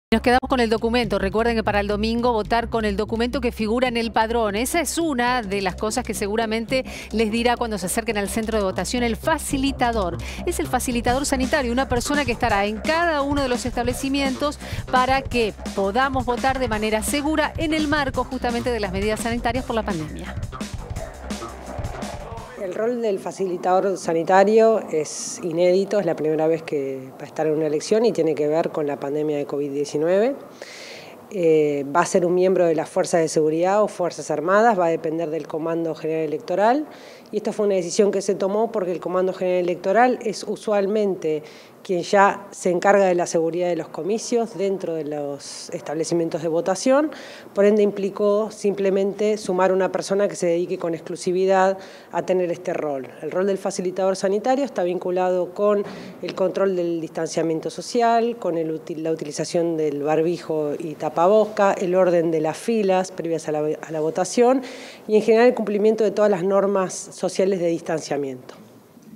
Cómo trabajarán los facilitadores sanitarios en las elecciones
Diana Quiodo, directora nacional electoral, explica cuáles serán las tareas que desplegarán estas personas para mantener los cuidados sanitarios que impone la pandemia.